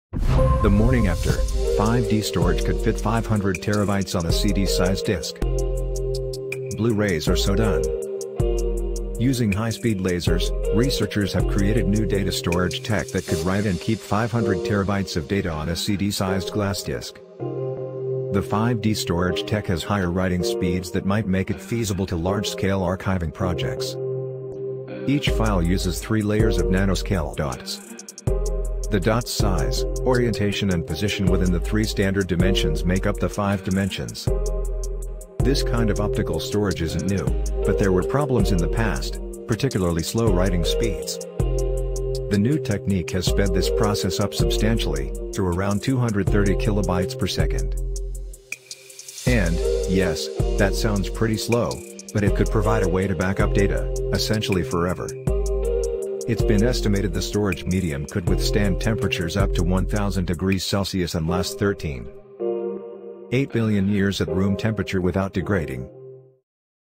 5D' storage could fit 500TB on a CD-sized disc
5D' storage could fit 500TB on a CD-sized disc